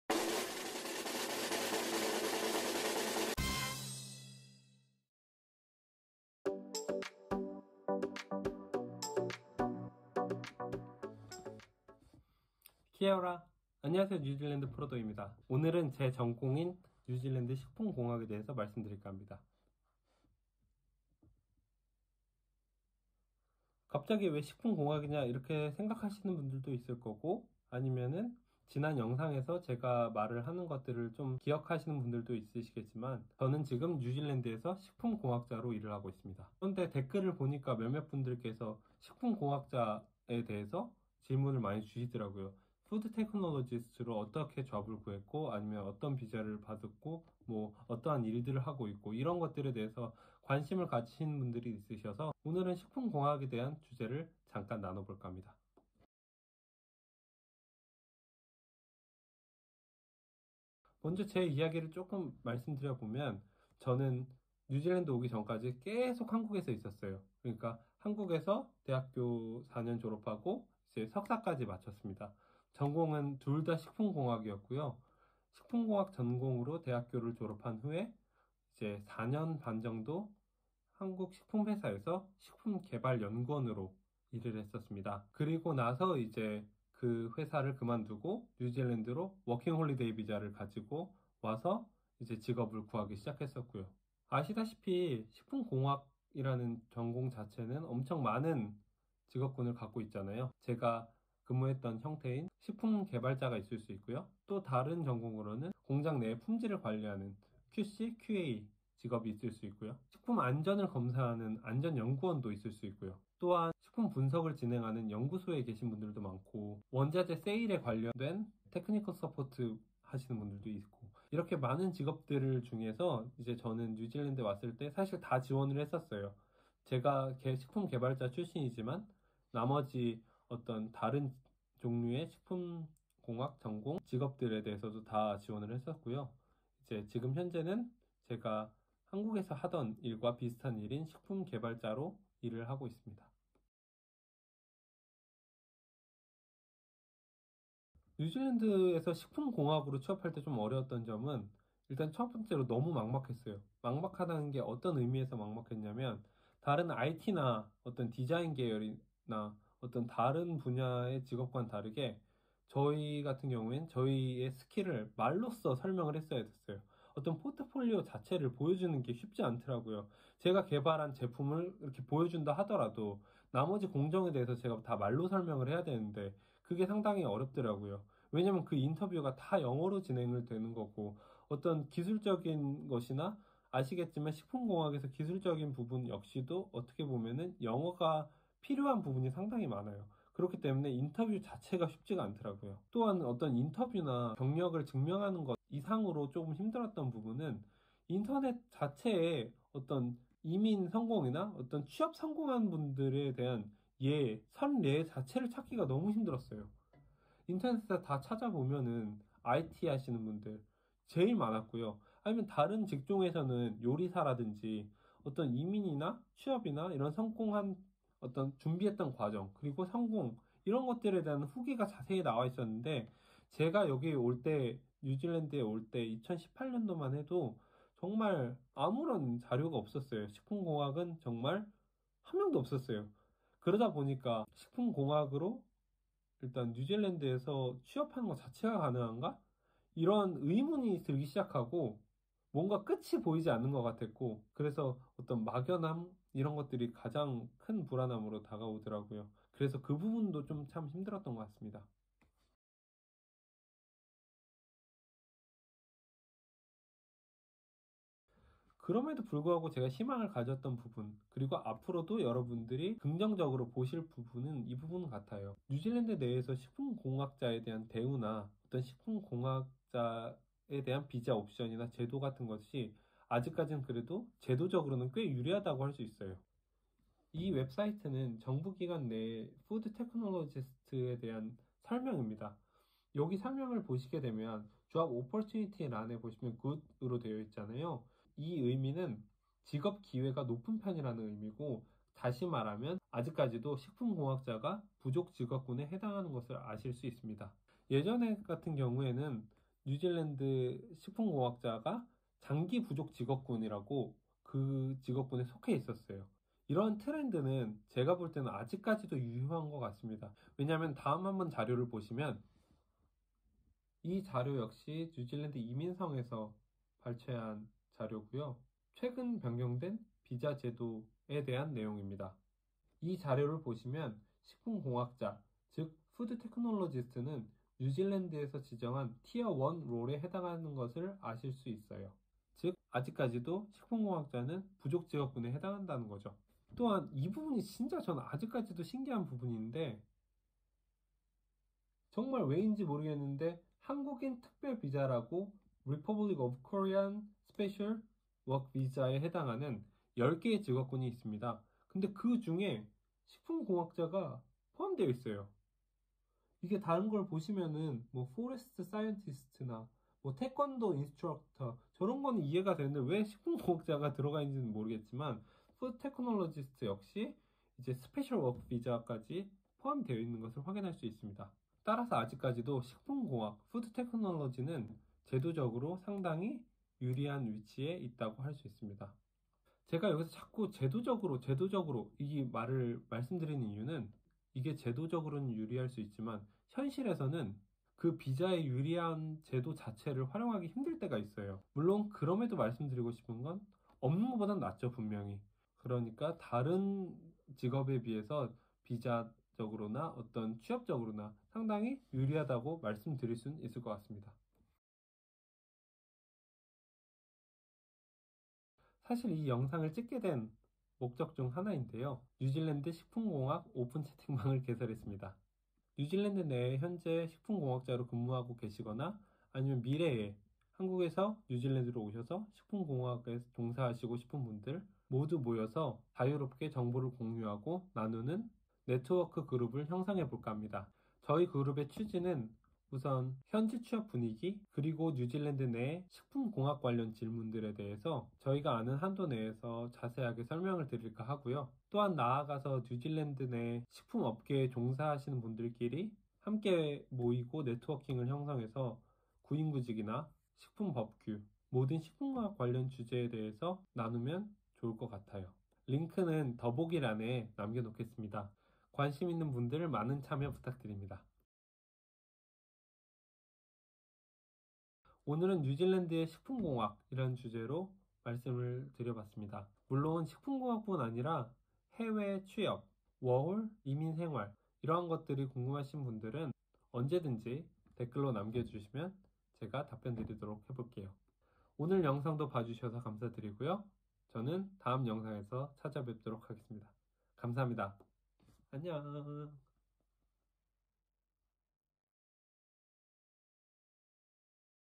🇳🇿뉴질랜드 정보/뉴질랜드 식품공학자(Food technologist) 모임을 개설했습니다!
안녕하세요,

오늘은 저의 전공인 식품공학에 대한 주제입니다.

제가 뉴질랜드에 오던 2018년에만 하더라도 식품공학, 즉 fopd technologist에 대한 정보가 너무 없어서 고생했던 기억이 납니다.

앞으로 오실 분들에게 조금이나마 도움을 드리고자
뉴질랜드 식품공학 모임을 개설했습니다!

관심 있으신 분들은 아래 링크로 참여하시면 됩니다 :)

감사합니다!